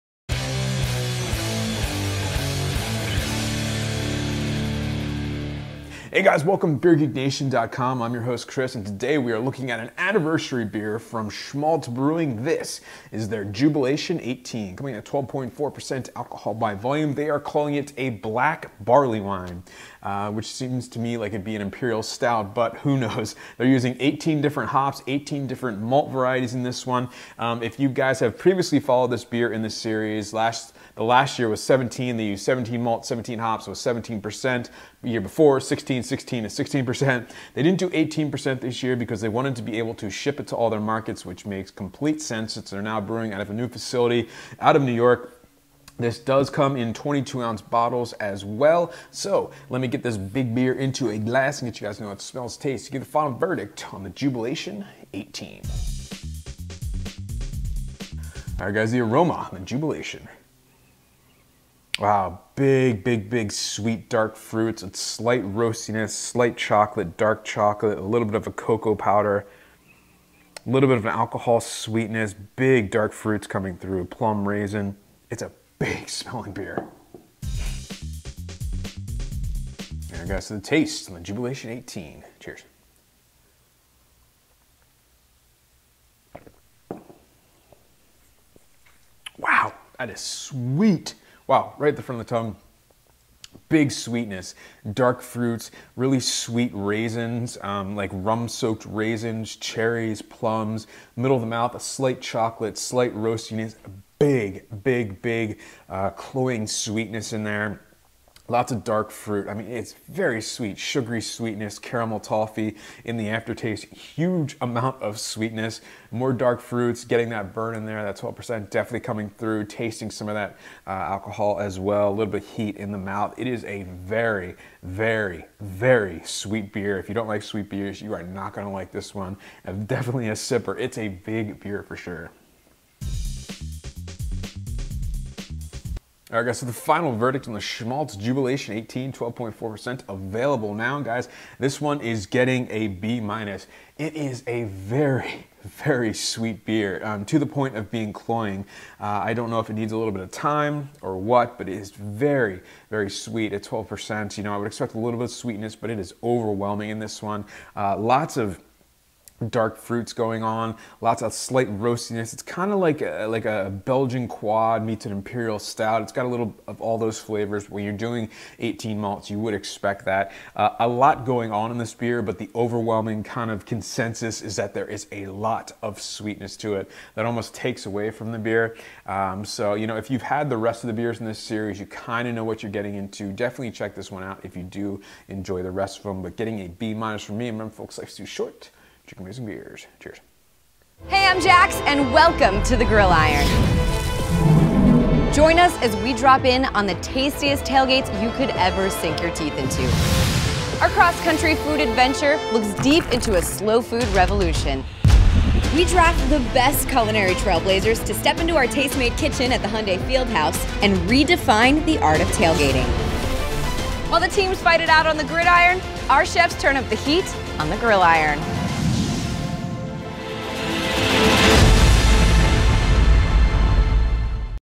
He’Brew Jewbelation Eighteen (12.4% ABV) | Beer Geek Nation Beer Reviews
Check out the new Tastemade Series The Grill Iron!
He’Brew Jewbelation Eighteen
Shmaltz Brewing Company
12.4%  ABV

Jewbelation 18® is the birth of an entirely new take on the series. When asked about this year’s Jewbelation®, Shmaltz founder Jeremy Cowan says, “So many friends have inquired about when will we stop upping the ante with the level of malts, hops, ABV, and my response has been ‘Never… well, um, I mean now!’ Witness our newborn black session barleywine. 18 malts, 18 hops, and now available in a delightfully sessionable 12.4% ABV so we can ship to allll our states again. Jewbelation is now in session. L’Chaim!”